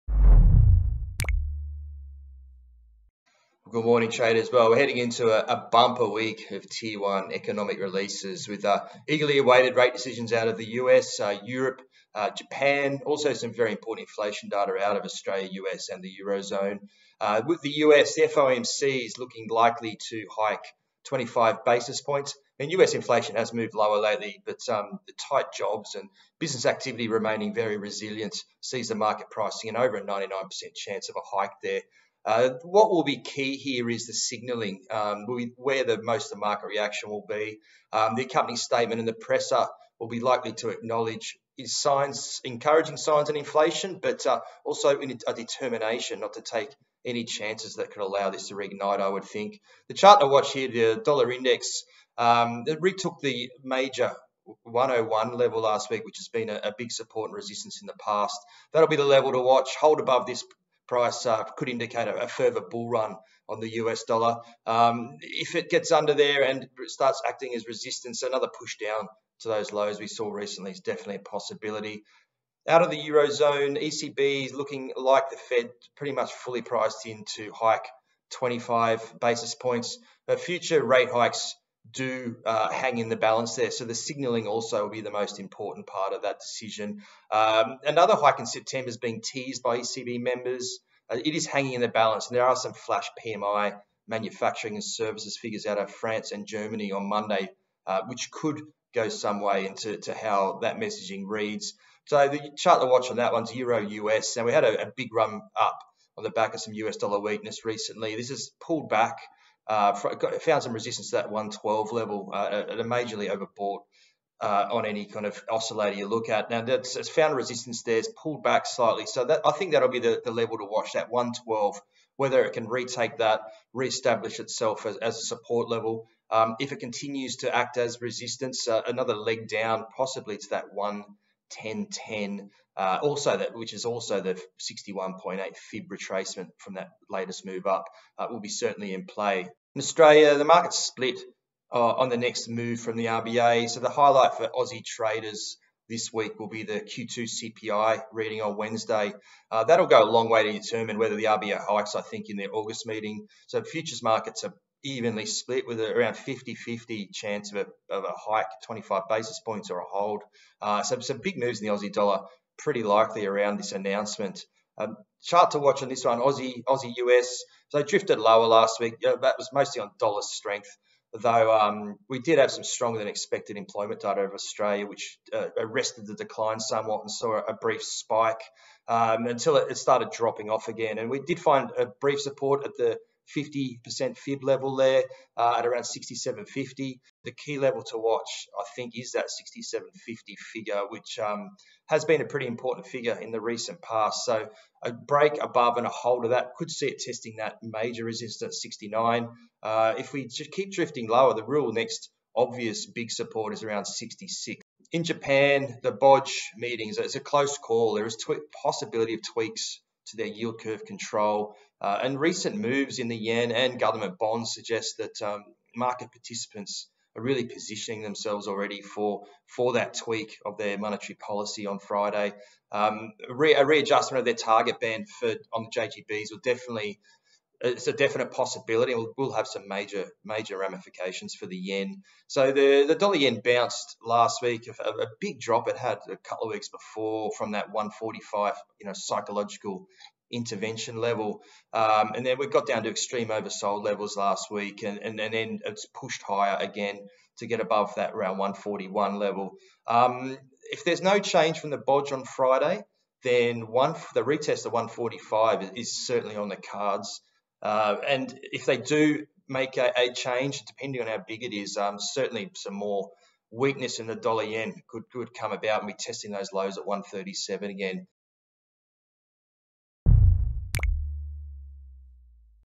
A pivotal week ahead in FX markets - the charts to watch
FOMC, ECB, BoJ rate decisions, Aussie CPI headline a massive week in markets.

Charts to watch